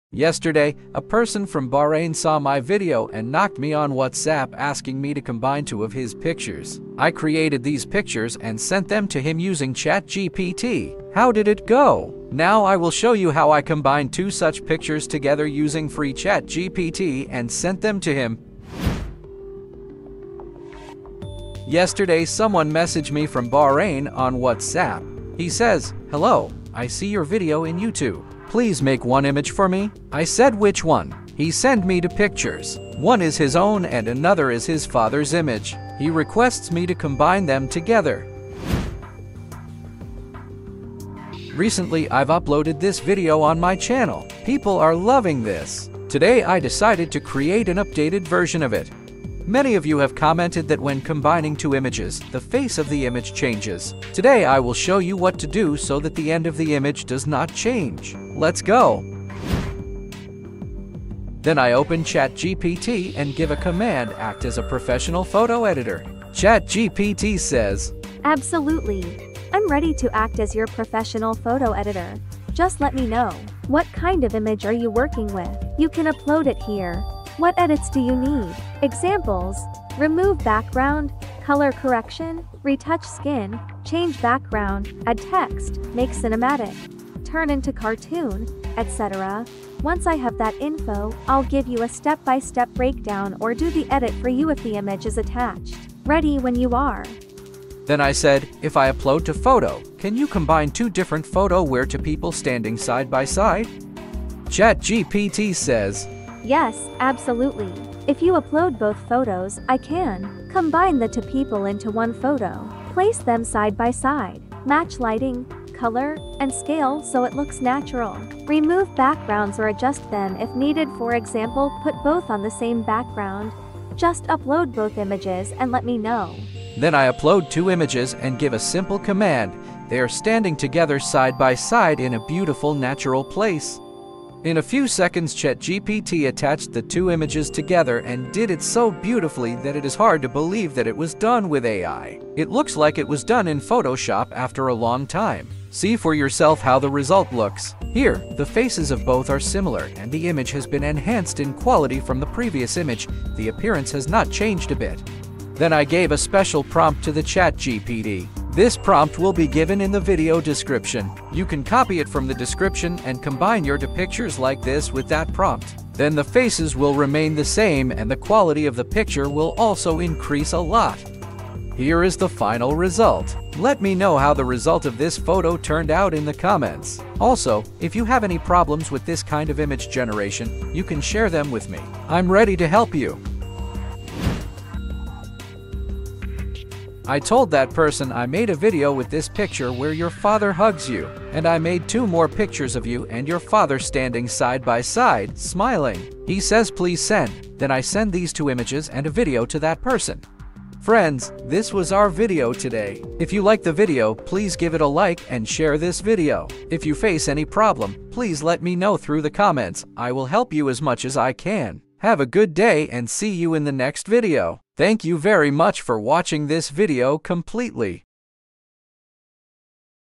This ChatGPT MAGIC PROMPT Merges 2 People into 1 Photo – 100% FREE!
This ChatGPT MAGIC PROMPT Merges 2 People into 1 Photo – 100% FREE! Want to merge two people into one realistic photo—using just a prompt and ChatGPT? In this step-by-step tutorial, I’ll show you how to generate stunning AI images where two faces blend into a single photo using a magic prompt with ChatGPT and free AI tools. No Photoshop, no technical skills required!

🧠 What You’ll Learn:
How to use ChatGPT to create a face merge prompt
Free tools that generate AI images from prompts
How to blend two people into one photo
Tips for making your AI photos look realistic

✨ Perfect for:
AI art lovers, content creators, digital artists, and anyone curious about creative uses of ChatGPT and AI image generators.

📌 Tools Used in This Video:
ChatGPT Free

👉 Try it yourself with the magic prompt below ⬇️

📋 Prompt shown at in the video: Create a realistic image by combining the two individuals from the provided separate photos into one single frame. Place both people side by side, standing close together as if they posed together for a photo. Ensure both individuals are looking straight at the camera and have natural, relaxed, smiling expressions. Match the lighting, skin tone, shadows, and perspective so that both individuals appear to be in the same environment. Use a single, visually appealing background like a softly lit indoor studio background that suits both subjects and keeps the focus on them. Adjust posture, alignment, and proportions so they look like close friends or family members, naturally positioned for a portra Avoid any harsh editing or mismatched lighting. Make sure the final image feels authenticDislike like it was taken in one shot. Make sure image charactor not changes.